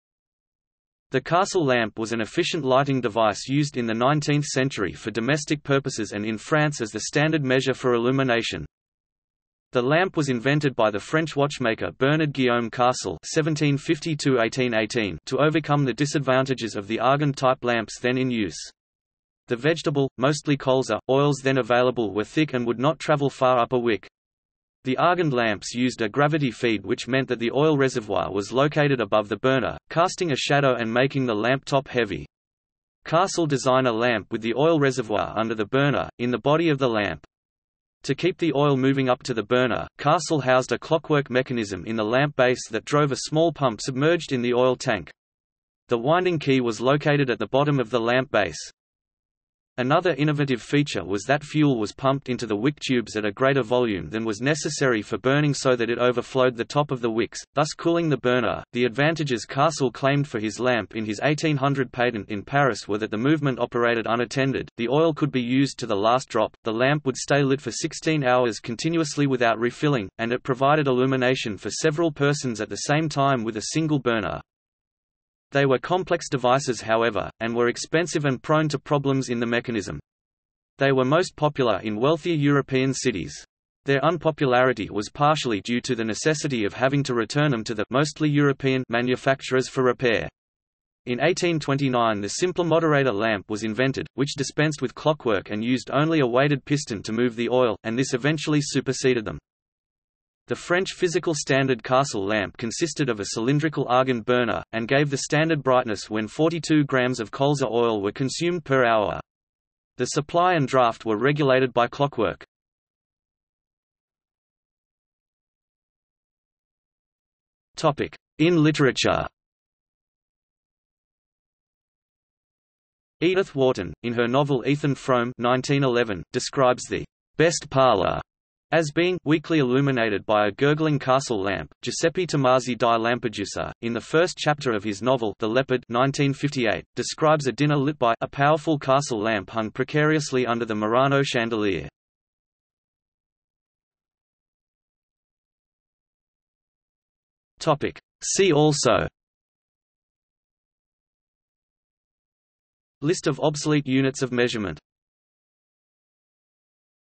Carcel lamp | Wikipedia audio article
00:01:54 In literature
00:02:23 See also

- Socrates

SUMMARY
The Carcel lamp was an efficient lighting device used in the nineteenth century for domestic purposes and in France as the standard measure for illumination.
The lamp was invented by the French watchmaker Bernard Guillaume Carcel (1750–1818) to overcome the disadvantages of the Argand-type lamps then in use. The vegetable - mostly colza - oils then available were thick and would not travel far up a wick. The Argand lamps used a gravity feed which meant that the oil reservoir was located above the burner, casting a shadow and making the lamp top heavy. Carcel designed a lamp with the oil reservoir under the burner, in the body of the lamp. To keep the oil moving up to the burner, Carcel housed a clockwork mechanism in the lamp base that drove a small pump submerged in the oil tank. The winding key was located at the bottom of the lamp base.
Another innovative feature was that fuel was pumped into the wick tubes at a greater volume than was necessary for burning so that it overflowed the top of the wicks, thus cooling the burner.The advantages Carcel claimed for his lamp in his 1800 patent in Paris were that the movement operated unattended, the oil could be used to the last drop, the lamp would stay lit for sixteen hours continuously without refilling, and it provided illumination for several persons at the same time with a single burner.
They were complex devices however, and were expensive and prone to problems in the mechanism. They were most popular in wealthier European cities. Their unpopularity was partially due to the necessity of having to return them to the (mostly European) manufacturers for repair. In 1829 the simpler Moderator lamp was invented, which dispensed with clockwork and used only a weighted piston to move the oil, and this eventually superseded them.
The French physical standard Carcel lamp consisted of a cylindrical Argand burner, and gave the standard brightness when 42 grammes of colza oil were consumed per hour. The supply and draught were regulated by clockwork.